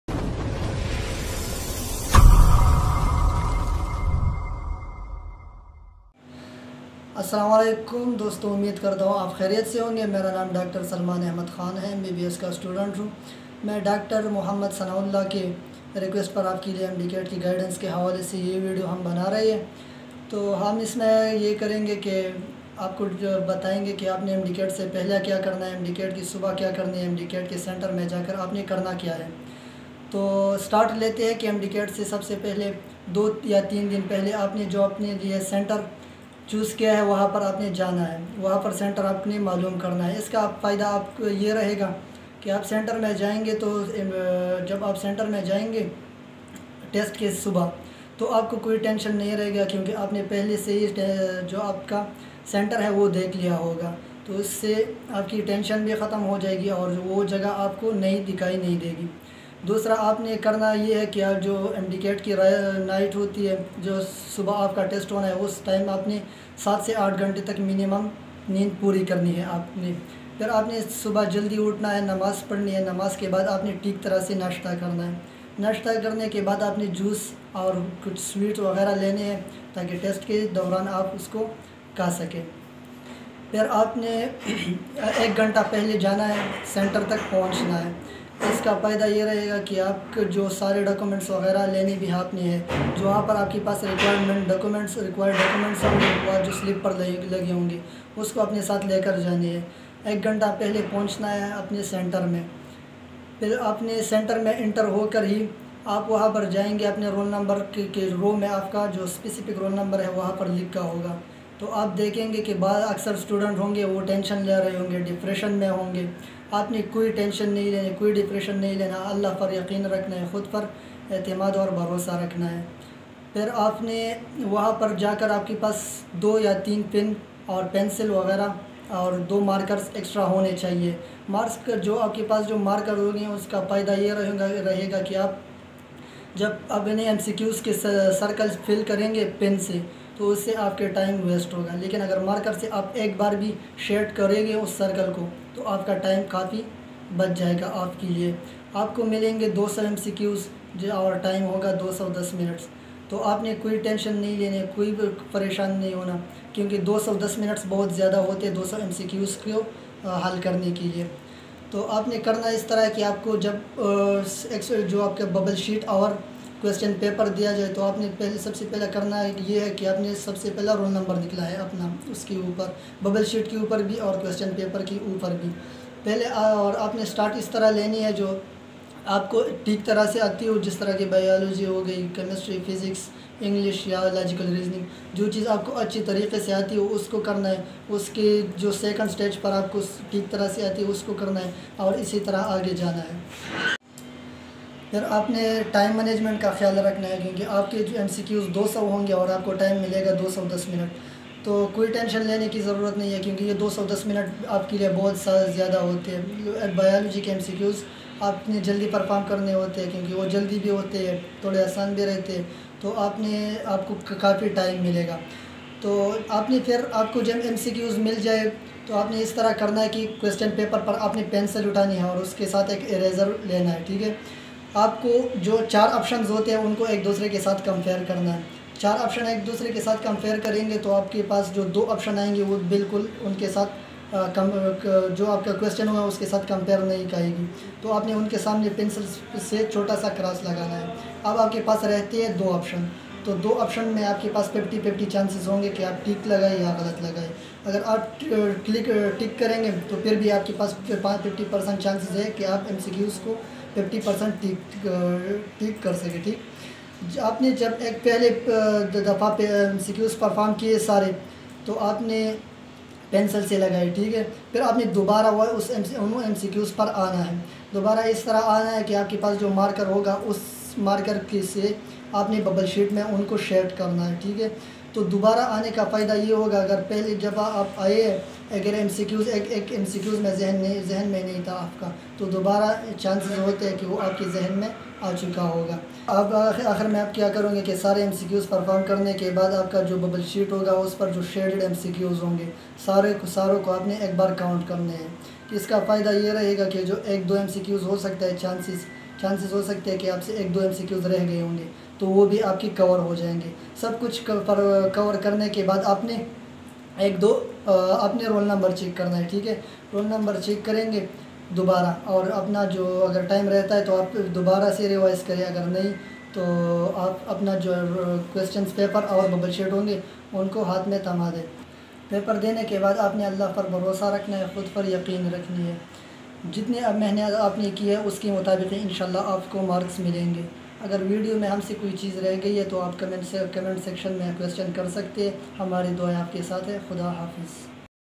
Last Two Days Before MDCAT|| What to do in exam center, Important instructions
Hi ! In this video Dr Salman Ahmed will guide you that what to do before 1/2 days of MDCAT exam and from which things you have to avoid.